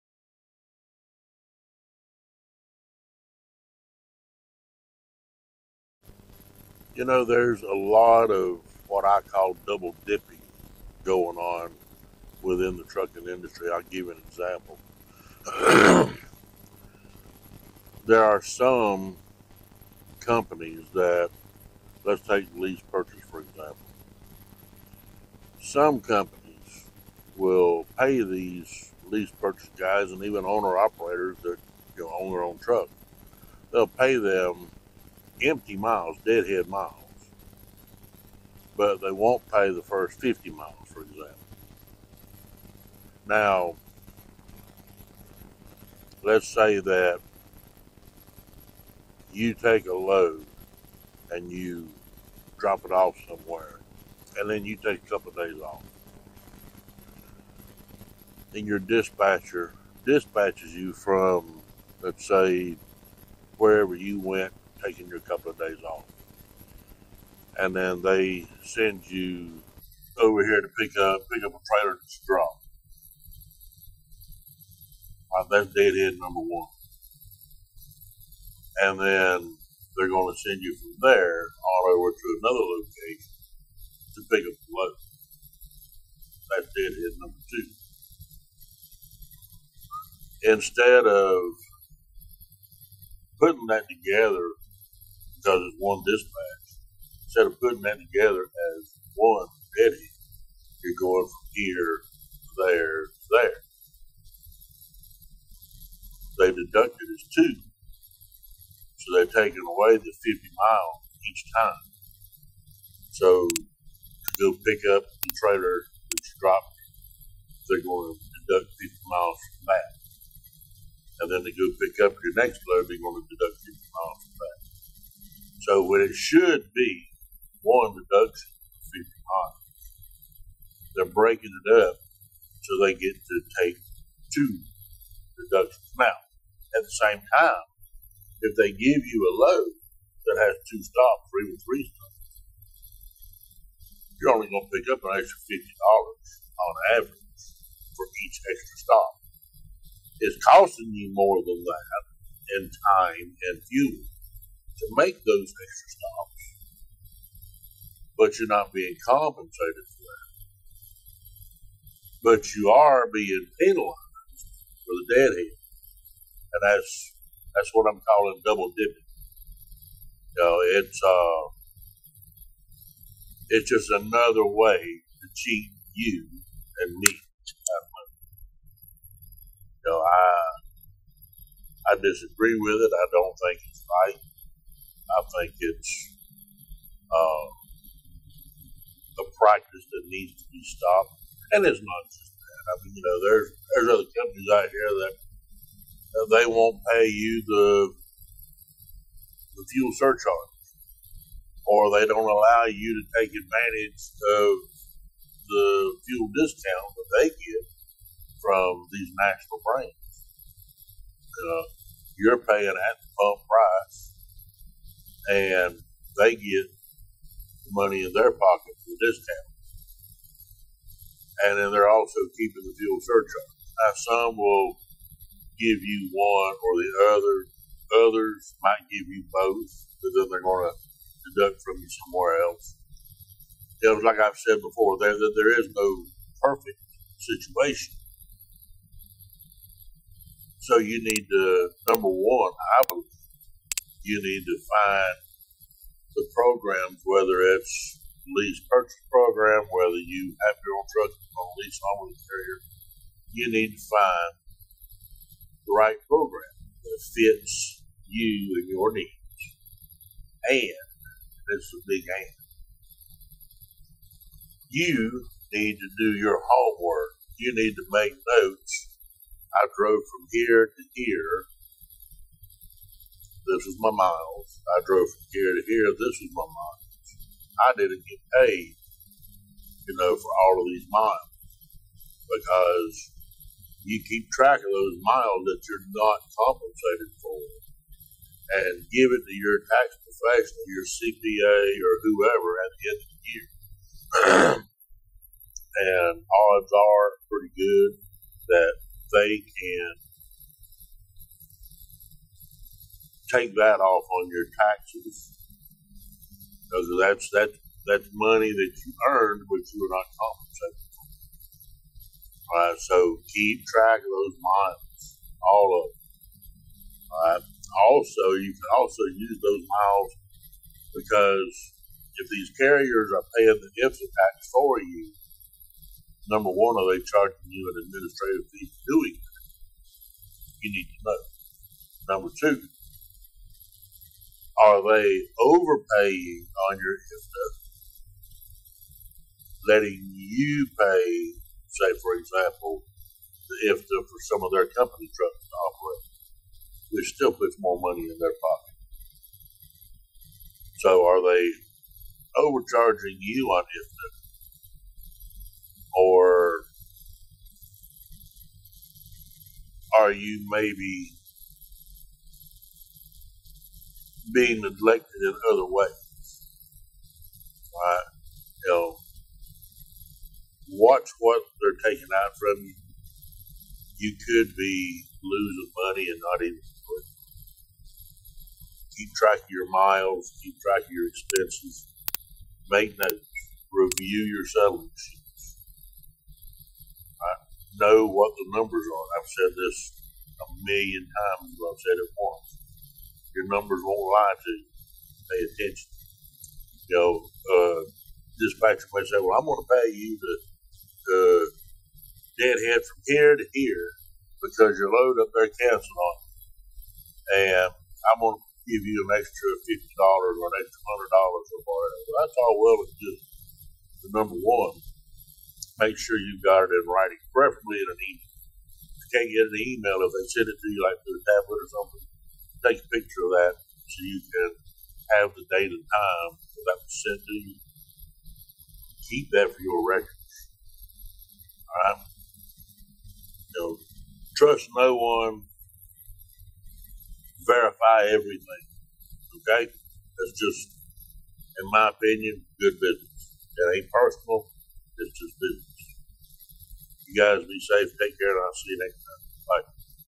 STOP Letting Them Take Your Money
Doesn't matter if you're an owner operator just leased onto a trucking company or in a lease purchase program, a lot of these carriers will try to sneak deductions by you or over charge you. We have to stay diligent and watch those settlement sheets. If we truck drivers don't hold them accountable no one else is going to. Take a listen to my video and let me know if this has happened to you. Stay safe and I'll see you down the road!